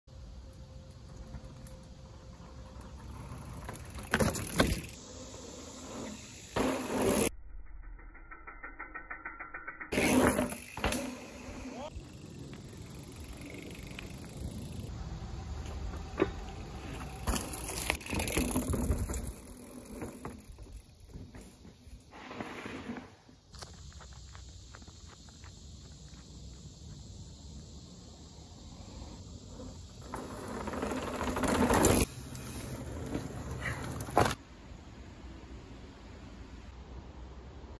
Palos Trail Fun
You can't beat the trail system out in the Palos Forest Preserves, Bullfrog Lake area, for good single track and interesting challenges along the way. Jumps, fallen trees to ride on, well built log piles and more.